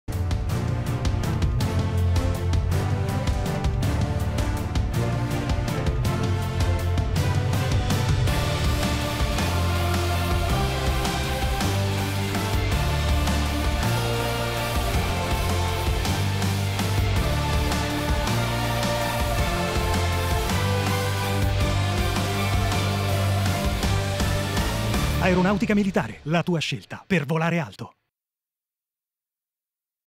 Campagna di comunicazione "Arruolamento Allievi Marescialli dell’Aeronautica Militare"
La campagna intende far conoscere le modalità di arruolamento nell’Aeronautica Militare, in particolare per il concorso per Allievi Marescialli. Inoltre vuole promuovere i valori connessi all’Istituzione e le diverse attività oggetto della formazione. Destinatari sono i giovani che abbiano un’età compresa tra i 17 e i 26 anni, che abbiano conseguito, o possano conseguire entro il 2021, il diploma di maturità e che abbiano l’idoneità psico-fisica al servizio militare.